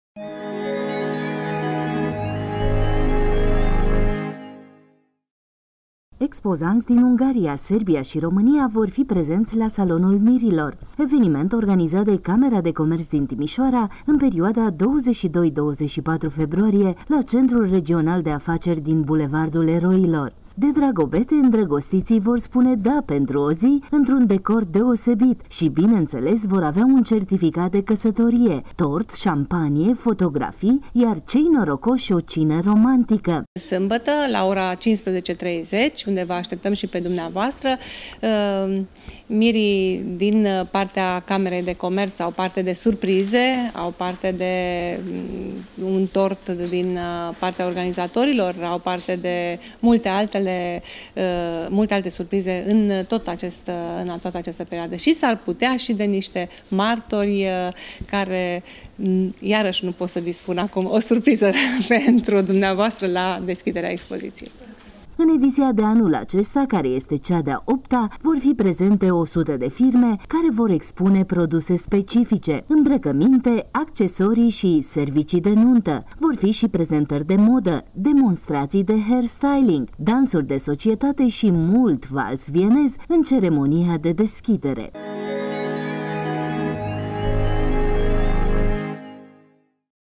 Casatoreste-te pentru o zi la Salonul Mirilor
Expozanti din Ungaria, Serbia si Romania vor fi prezenti la Salonul Mirilor, eveniment organizat de Camera de Comert din Timisoara, in perioada 22 - 24 februarie la Centrul Regional de Afaceri din Bulevardul Eroilor.

De Dragobete, indragostitii vor spune "DA",pentru o zi, intr-un decor deosebit si bineinteles vor avea un certificat de casatorie, tort, sampanie, fotografii si cei norocosi o cină romantică.

In editia de anul acesta care este a 8-a, vor fi prezente 100 de firme care vor expune produse specifice: imbracaminte, accesorii si servicii de nunta. Vor fi si prezentari de moda, demonstratii de hair styling, dansuri de societate si mult vals vienez in ceremonia de deschidere.